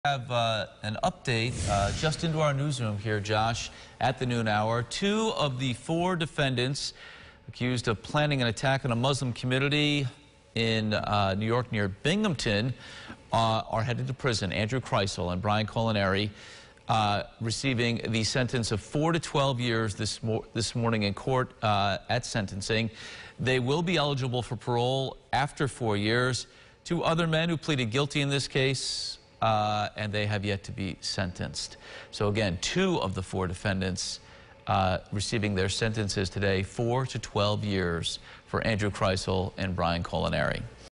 Two accused of planning to attack Muslim community sentenced
They were both sentenced to four to 12 years.